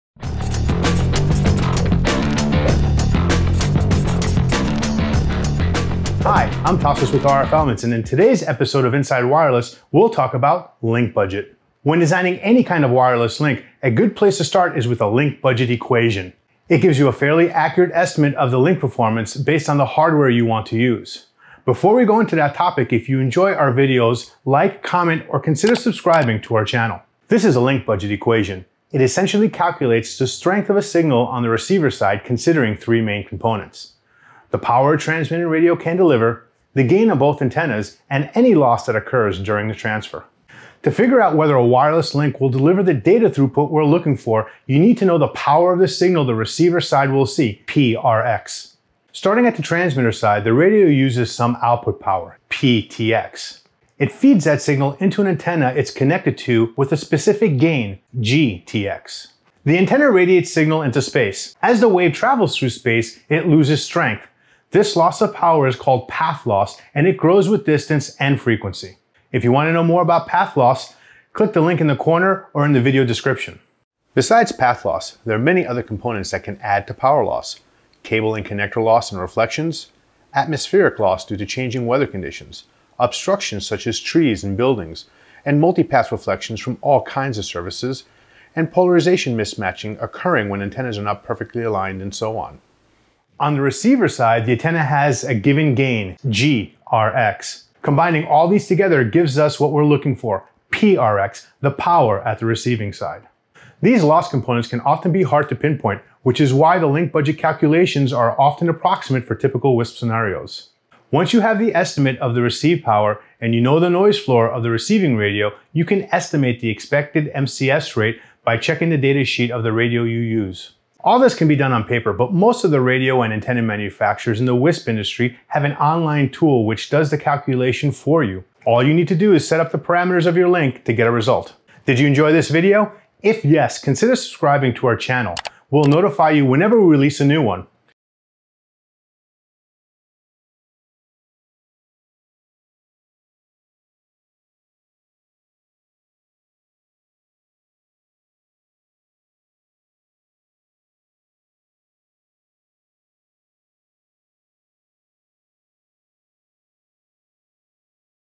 Inside Wireless: Link Budget
Alpha and omega of every wireless link planning is Link budget equation. How to use it? What are all the components to consider? In this Inside Wireless video, you'll learn all about Link Budget. Try RF elements Link Calculator to see it in practice

The equation essentially calculates the power for an RF signal on the receiver side considering three main components:
- Power provided by the transceiver radio
- Gains of antennas on both sides of a link
- Any loss that occurs during the transfer

- cables + connectors introduce power loss and reflection
- atmospheric conditions dissipate the wave power depending on the frequency of the signal
- multipath reflections cause random attenuation
- polarization mismatch results into power loss as well

In reality, parts of the loss can be often neglected while still maintaining good accuracy of the calculation. Naturally, the biggest loss factor is the most important, so if a cable loss is a fraction of dB, it can be ignored.

Nevertheless, the calculation of link budget is always an estimate that can be done manually on paper, but most of the manufacturers in the WISP industry have a link calculator that does the math for you with varying level of detail - some include trees, maybe weather, but very few include multipath propagation due to the computational intensity when modeling it.

0:00 introduction
0:12 The equation
1:04 Loss components
1:51 Loss & MCS rate connection
2:00 Link calculator